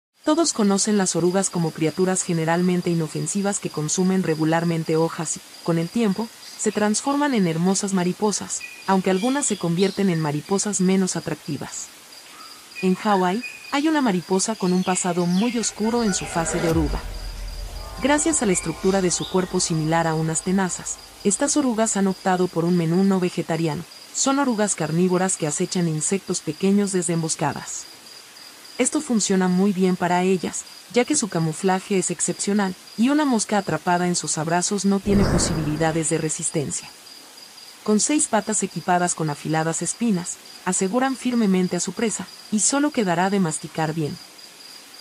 Oruga depredadora con patas.
"Eupithecia orichloris" Eupithecia orichloris es una especie de polilla de la familia Geometridae, originaria de Kauai, Oahu, Maui, Lanai y Hawái. Las larvas u orugas de esta especie y de unas pocas especies relacionadas son depredadores que se alimentan de otros insectos.
Todos conocen las orugas como criaturas generalmente inofensivas que consumen regularmente hojas y, con el tiempo, se transforman en hermosas mariposas, aunque algunas se convierten en mariposas menos atractivas. En Hawái, hay una mariposa con un pasado muy oscuro en su fase de oruga. Gracias a la estructura de su cuerpo similar a unas tenazas, estas orugas han optado por un menú no vegetariano; son orugas carnívoras que acechan insectos pequeños desde emboscadas. Esto funciona muy bien para ellas, ya que su camuflaje es excepcional, y una mosca atrapada en sus abrazos no tiene posibilidades de resistencia. Con seis patas equipadas con afiladas espinas, aseguran firmemente a su presa, y solo quedará de masticar bien.